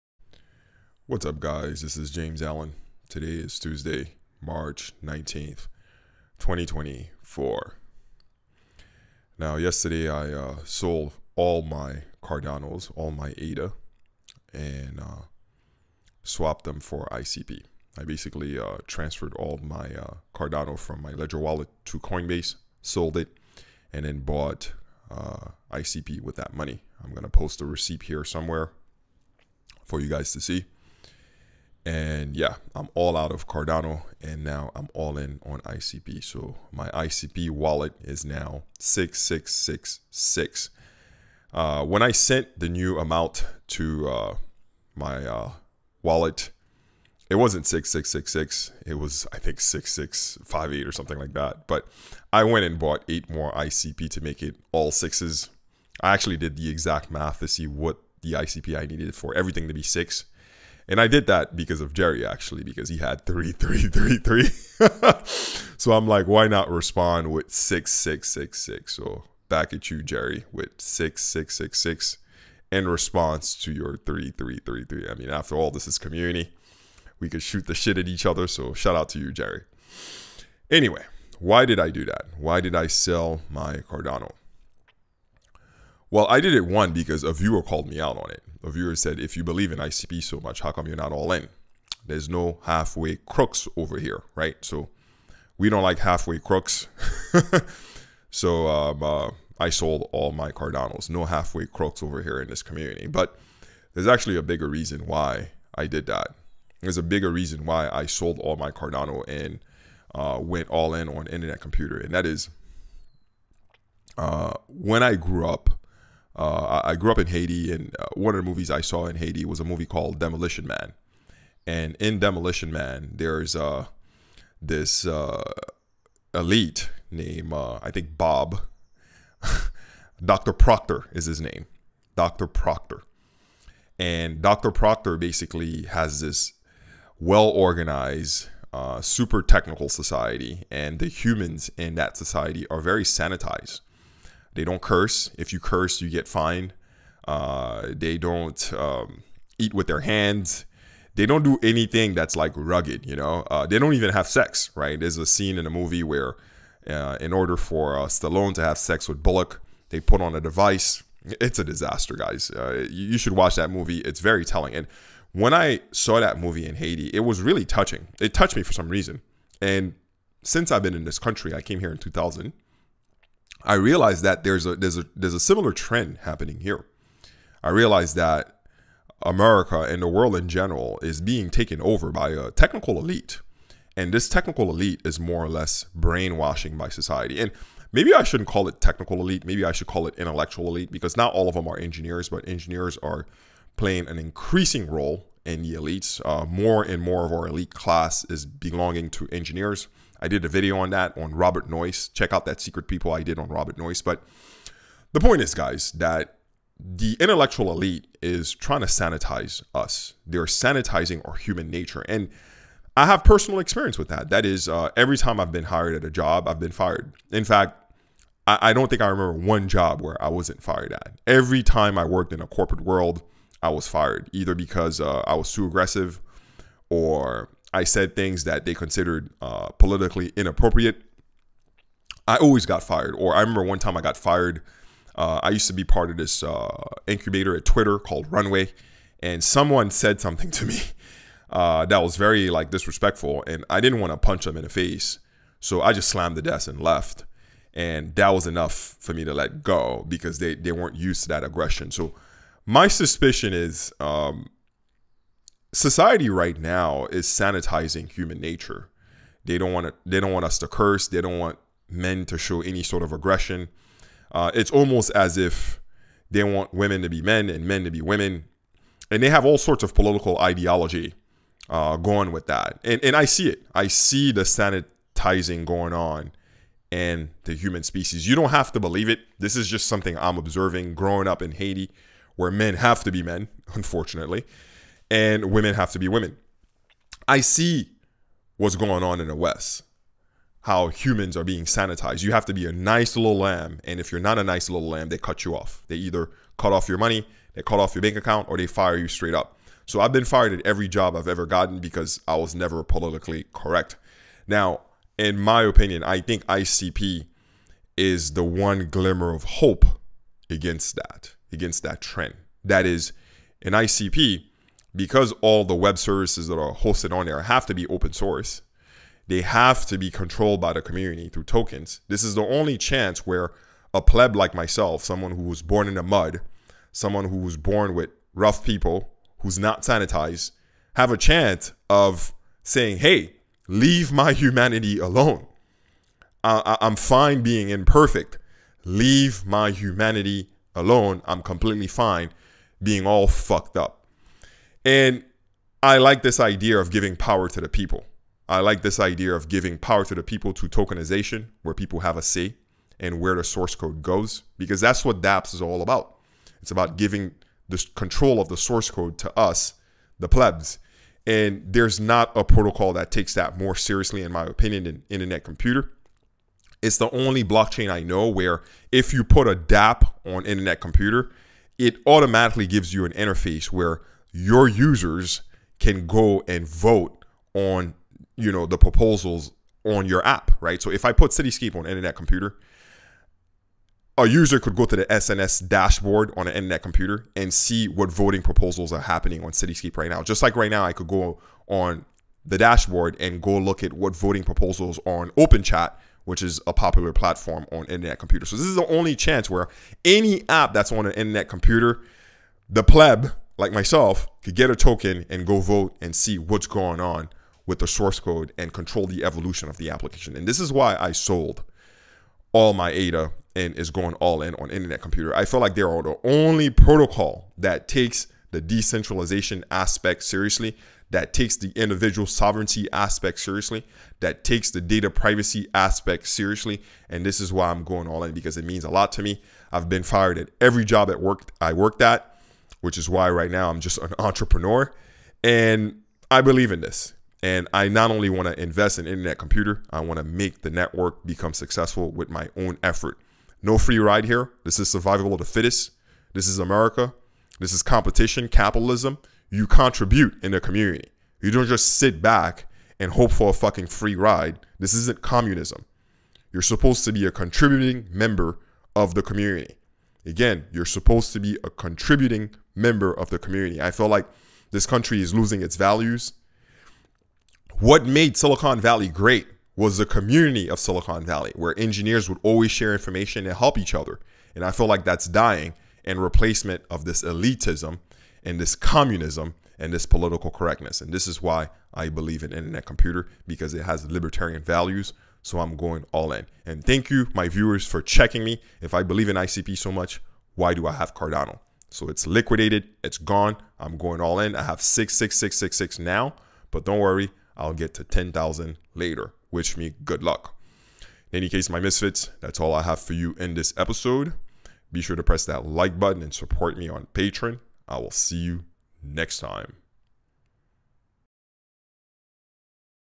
I Sold all my ADA for ICP
A viewer had previously called me out on the fact that my Cardano wallet was much heavier than my Internet Computer wallet. How can that be if I am a true believe of ICP? There are no halfway crooks in this community and I will definitely not set the wrong example. In this episode, I explain why I sold my Cardano positions and why I have decided to go all in on Internet Computer. Hope you enjoy this candid episode!